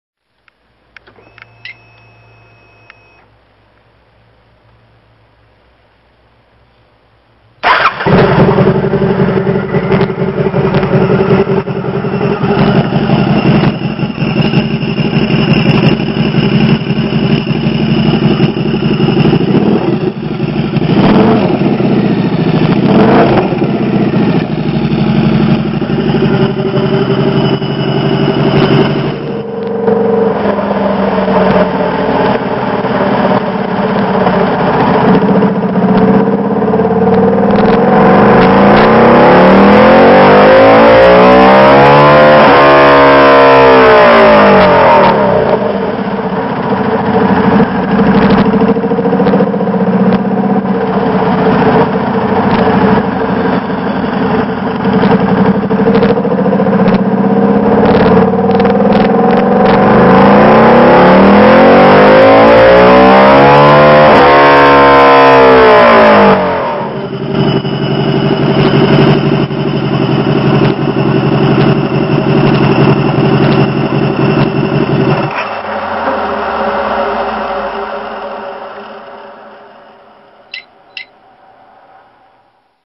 VRod Turbo Dyno Runs...Custom mapped by kcadby...
Screamin Eagle VRod with Trask Turbo System...
Dyno Runs after Custom Mapping by kcadby with SE Race Tuner...